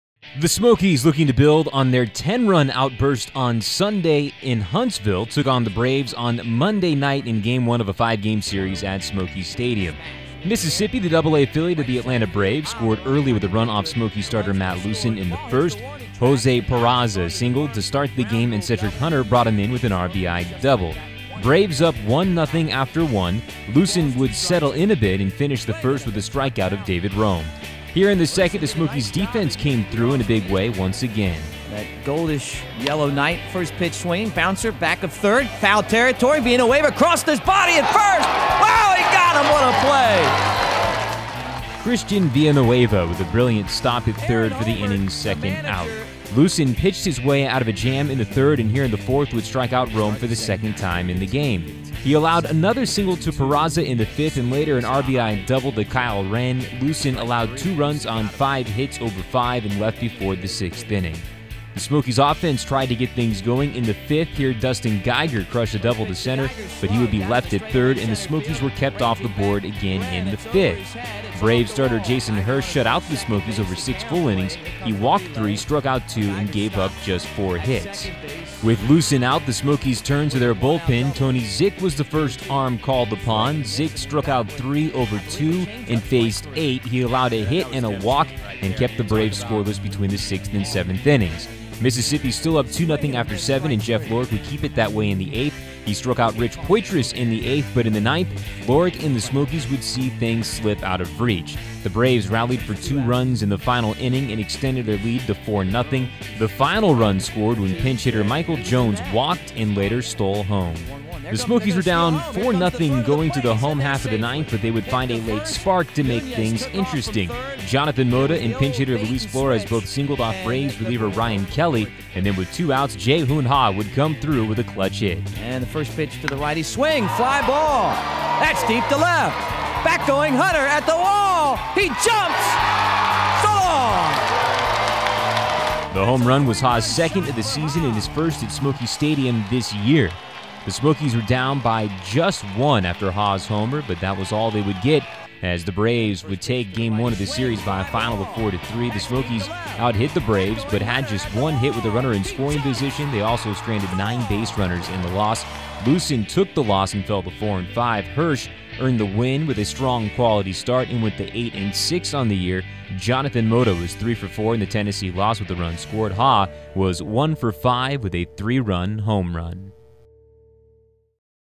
July 21 - Smokies' Comeback Thwarted In 4-3 Defeat
Mississippi starting pitcher Jason Hursh shut down Tennessee batters with six shutout frames while inducing three double plays and second baseman Jose Peraza tallied three hits and three steals, while Smokies center fielder Jae-Hoon Ha's ninth-inning three-run homer was too little too late in the Braves' 4-3 victory on Monday evening at Smokies Stadium.